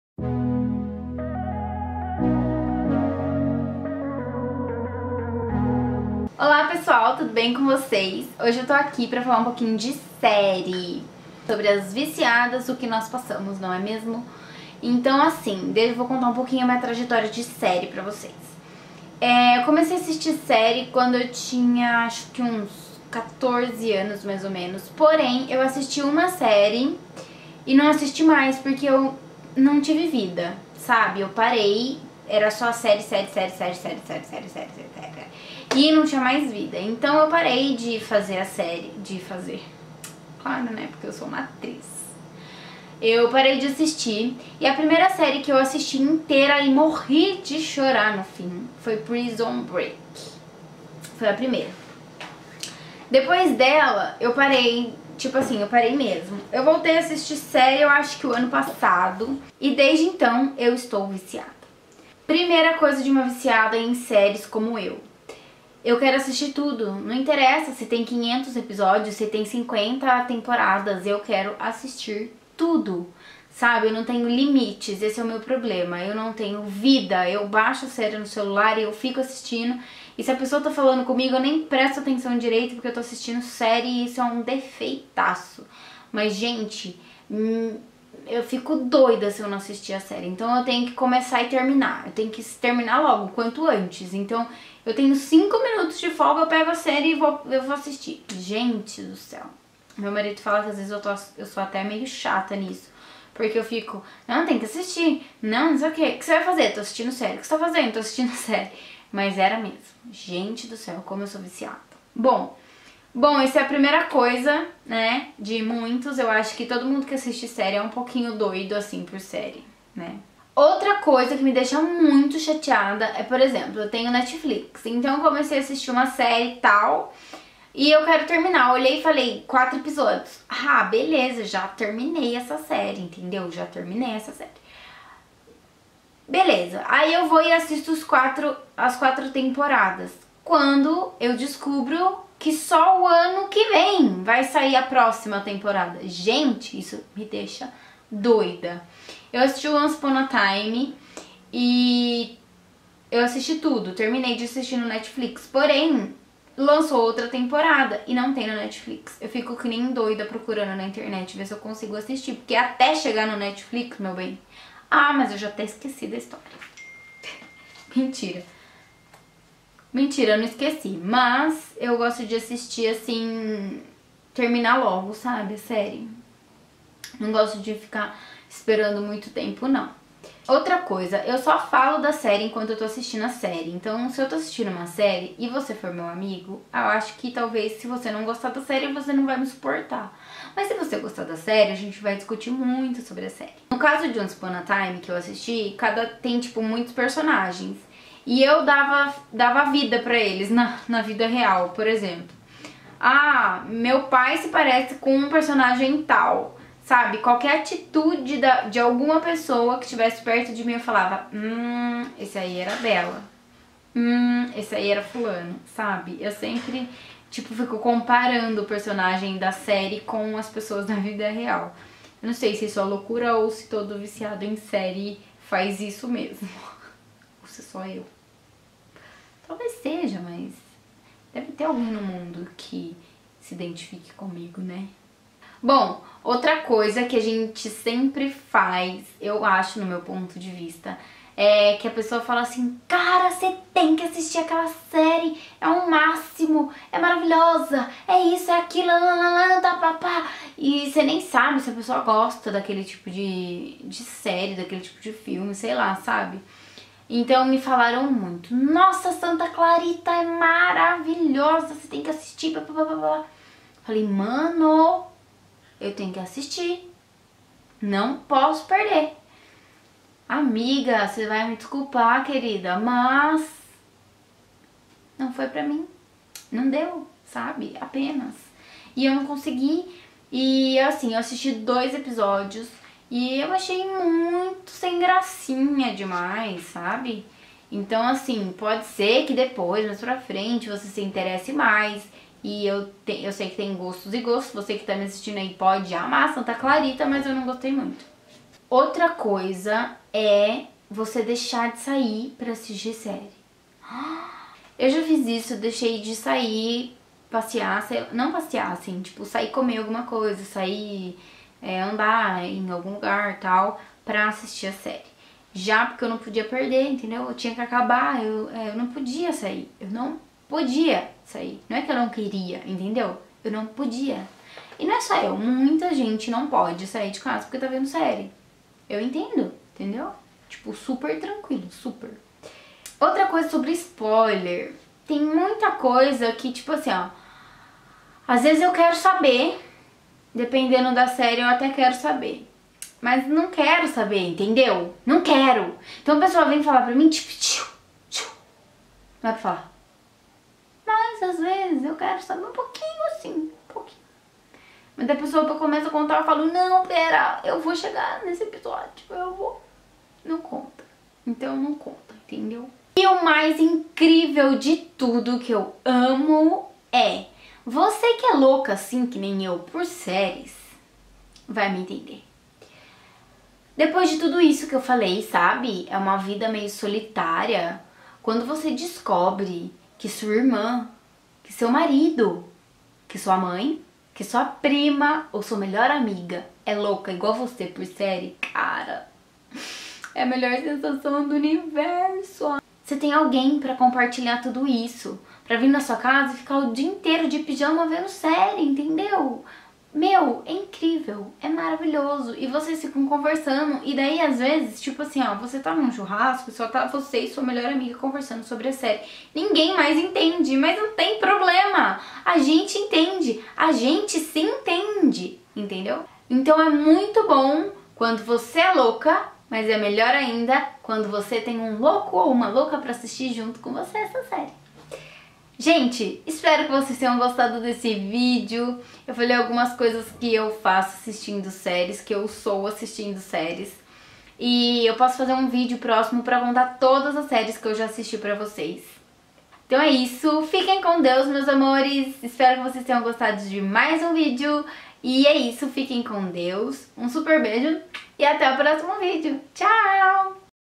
Coisas que uma viciada por séries faz!
Sei que tem muitos viciados em séries, e eu sou um deles, mostrei algumas coisas que faço quando assisto séries, espero que gostem, beijos.